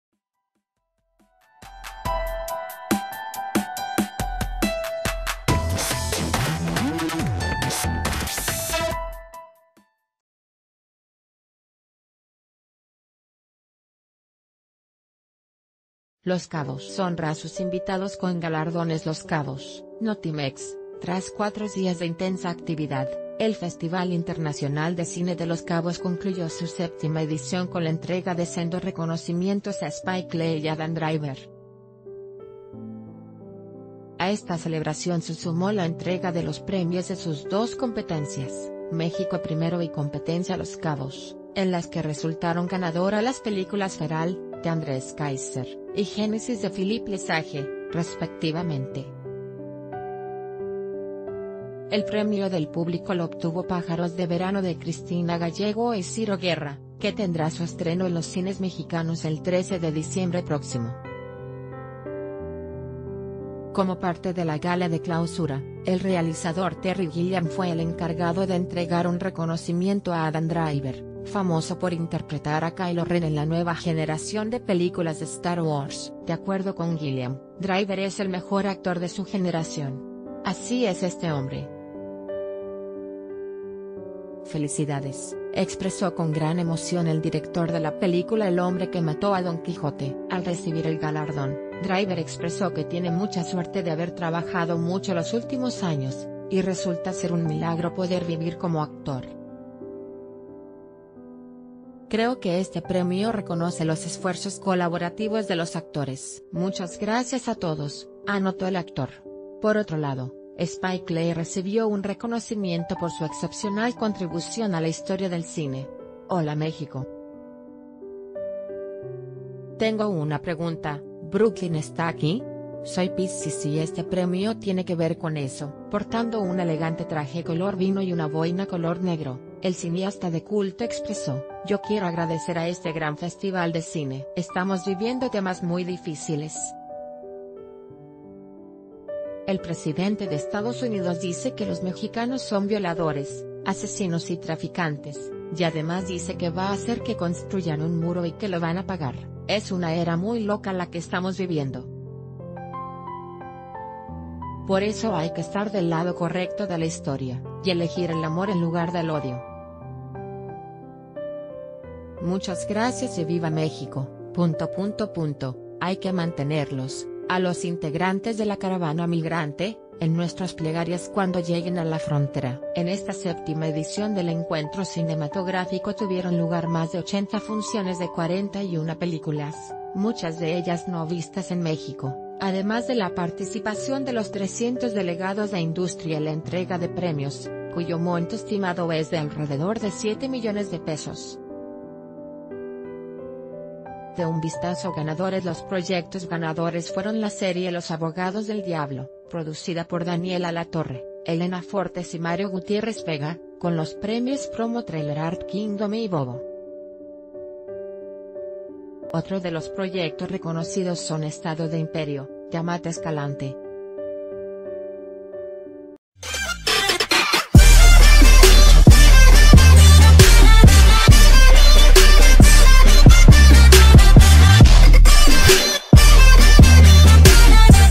Concluye el festival de Los Cabos - El Diario de Yucatán
Los Cabos honra a sus invitados con galardones LOS CABOS (Notimex).— Tras cuatros días de intensa actividad, el Festival Internacional de Cine de Los Cabos concluyó su séptima edición con la entrega de sendos reconocimientos a Spike Lee y Adam Driver. A esta celebración se sumó la entrega de los pre...